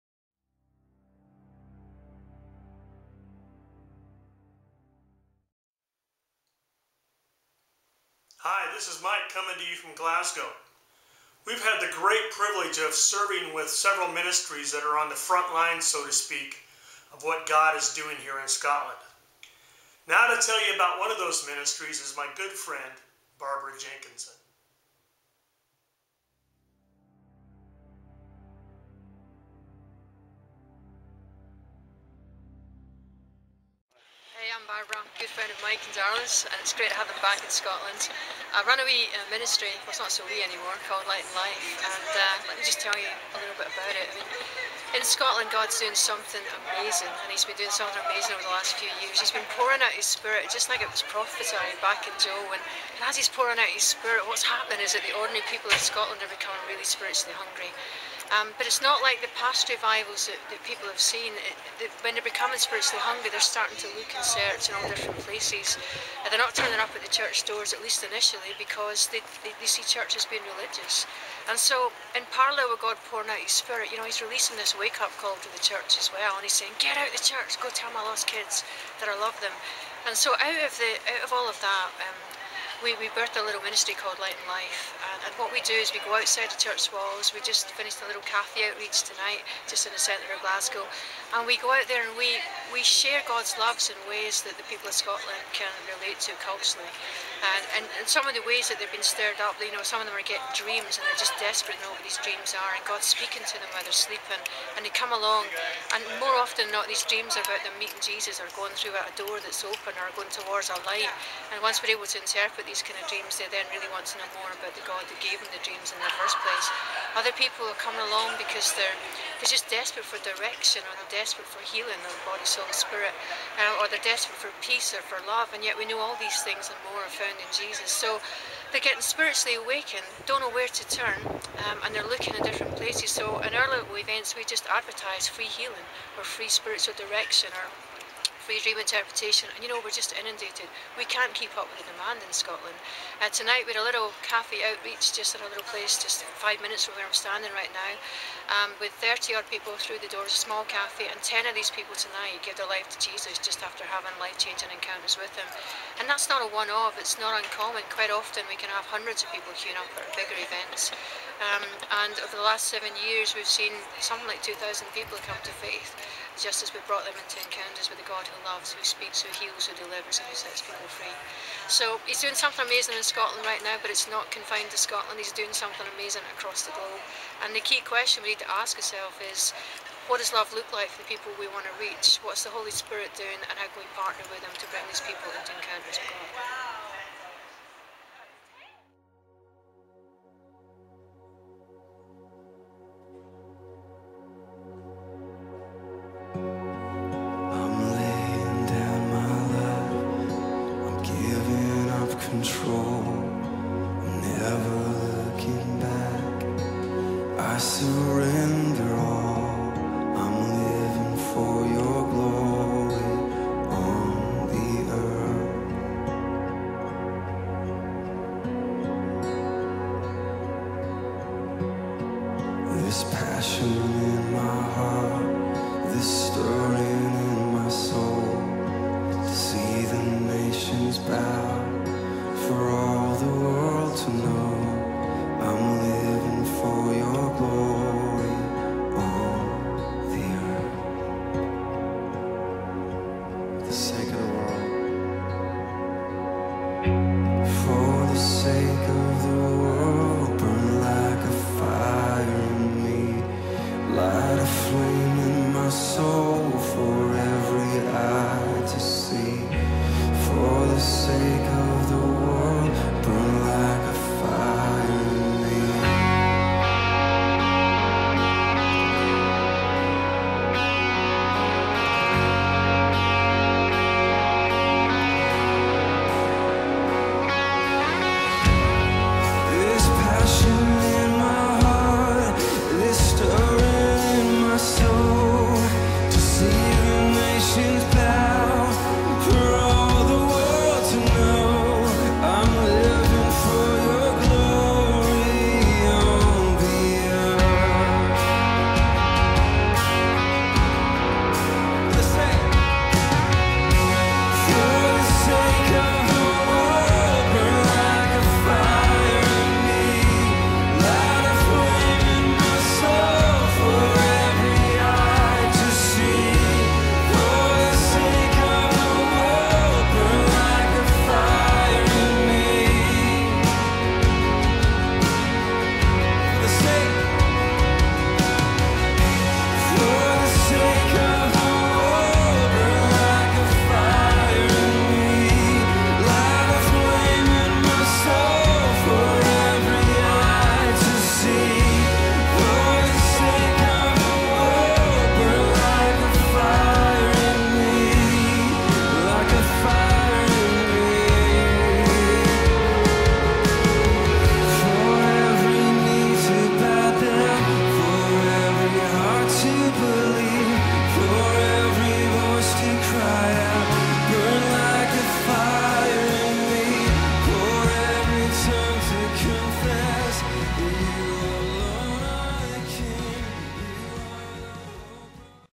The Outreach Ministry of Light & Life - Burning Hearts Ministries
This is about a powerful ministry in Scotland called Light & Life. We have had the great privilege of working with them during our time in the nation. Mike